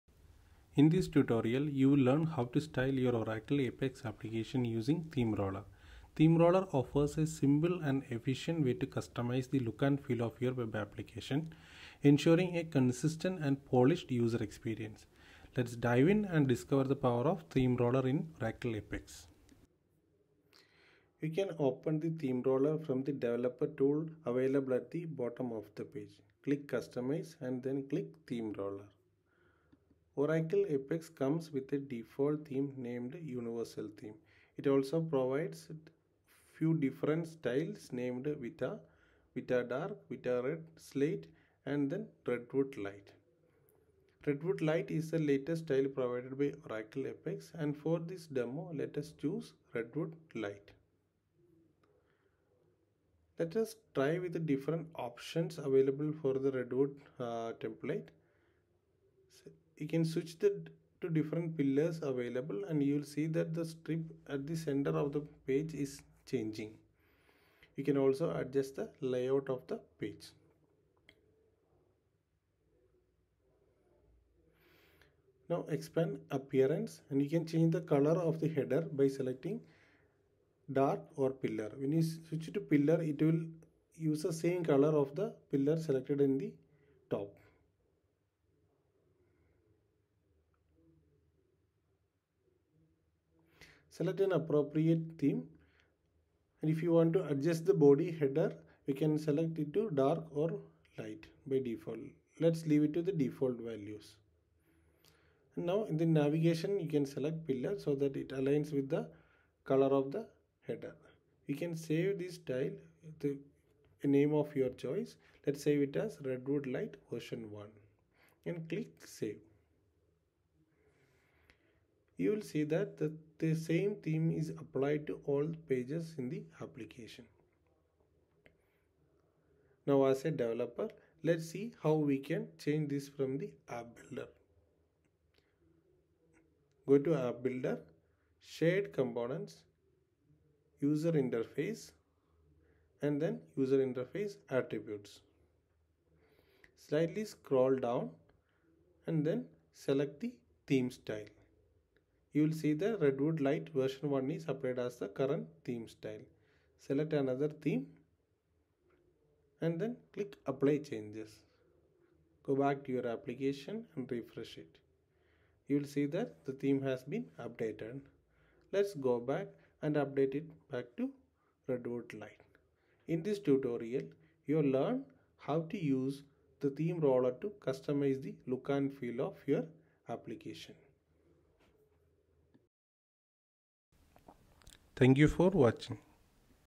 Elevate Your Oracle APEX App: Styling with Theme Roller for Professional and Consistent Designs
Learn how to style your Oracle APEX application with ease using Theme Roller. Customize the look and feel of your web app, apply themes, and tweak design elements to create a visually appealing user interface. Join us in this tutorial as we explore the power of Theme Roller in Oracle APEX and elevate the aesthetics of your application. Enhance user experience and make a lasting impression with beautifully styled Oracle APEX apps. Watch now and level up your design game with Theme Roller!